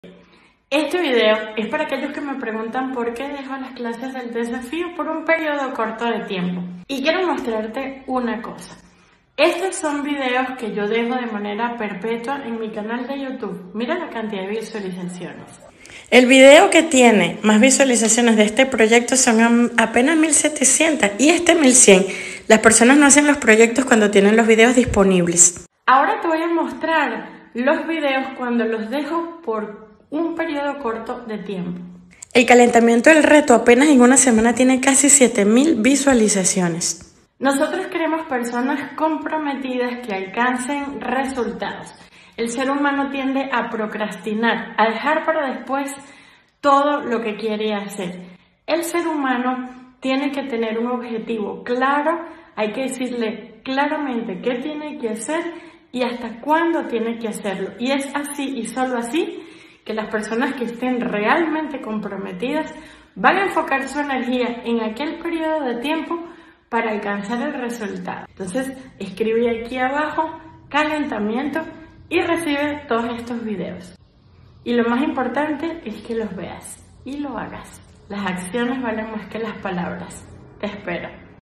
Por qué dejo los videos del Desafío por poco tiempo…
🚨 Escribe “Calentamiento” abajo 👇🏽 y lee los motivos por los que dejamos las clases al aire por poco tiempo 🪷🪷🪷🪷

Foco en el Objetivo: Las personas tienden a cumplir sus objetivos cuando se les indica con claridad que hacer y hasta cuando tienen para hacerlo!

Combatimos la procrastinación: tendemos a dejar todo para después y se nos acumulan y acumula todo en la lista de “por hacer”. Colocando una fecha tope, hay una tendencia mayor a que las personas se comprometan y hagan los mandalas!

Concentración de energía: cuando tenemos una fecha determinada para terminar una actividad concentramos mucho más nuestra intención y nos beneficiamos del proceso de crear Mandalas!

Si quieres ver las clases del calentamiento que estarán disponibles hasta el 20/02 escribe “calentamiento”